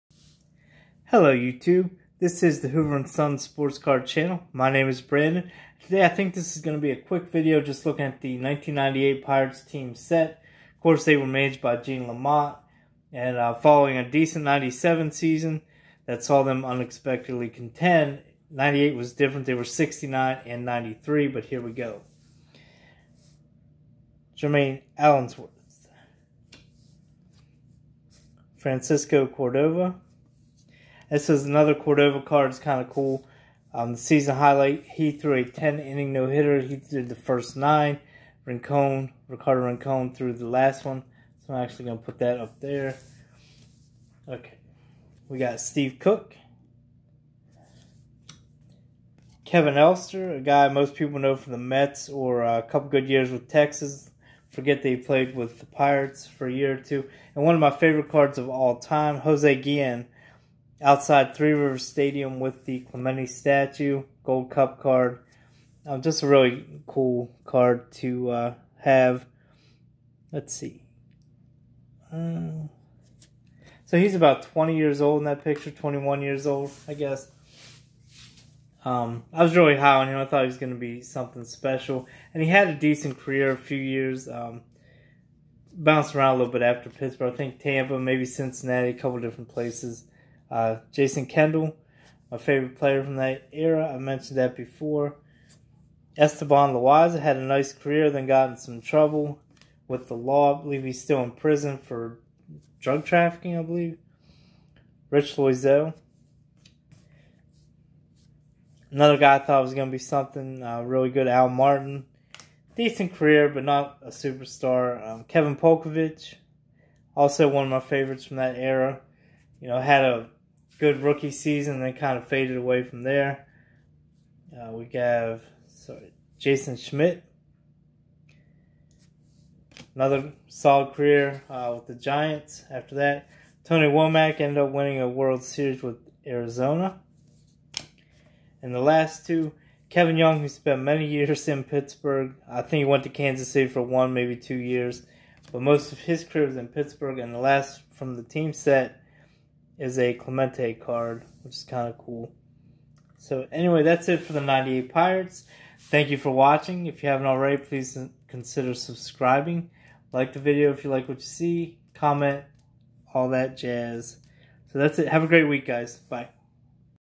1998 Pittsburgh Pirates Topps team set
Today, we are looking at the 1998 Pirates Topps team set.